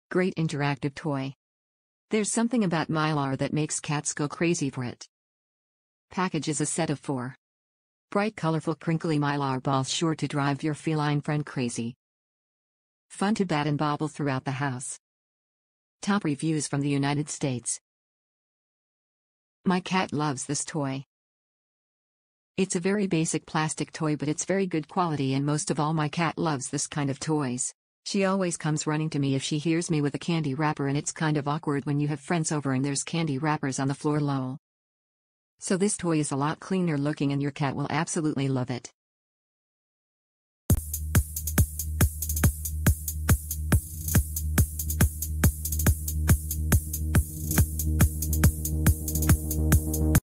HDP Spot Mylar Balls Cat Toys
Great interactive toy
There's something about mylar that makes cats go crazy for it
Package is a Set of 4
Bright colorful crinkly mylar balls sure to drive your feline friend crazy. Fun to bat and bobble throughout the house.

My cat loves this toy!!!

It’s a very basic plastic toy but it’s very good quality and most of all my cat loves this kind of toys !! She always comes running to me if she hears me with a candy wrapper and it’s kind of awkward when you have friends over and there’s candy wrappers on the floor lol. So this toy is a lot cleaner looking and your cat will absolutely love it.